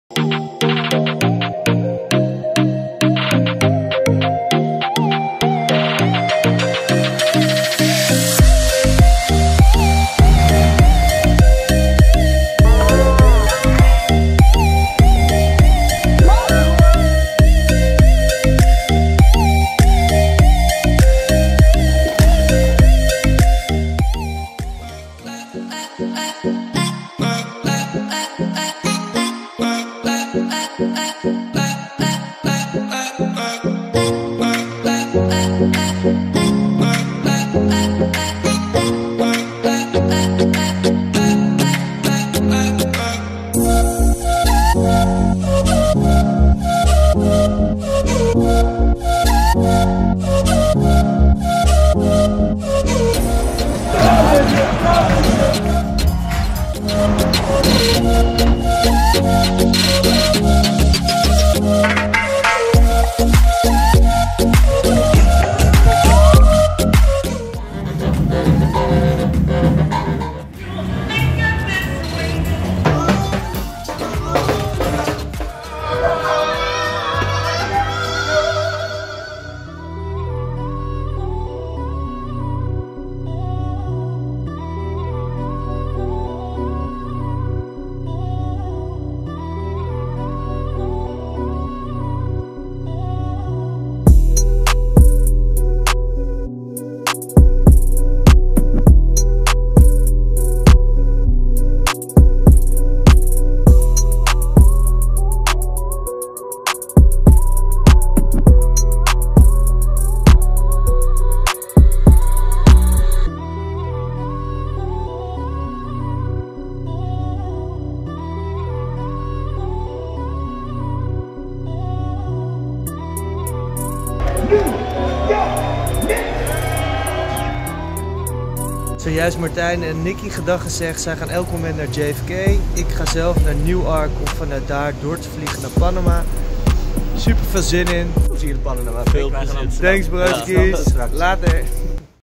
Fabian Muller - Travel Video - 10 days NewYork highlights
After this trip I decided to move by myself to Panama.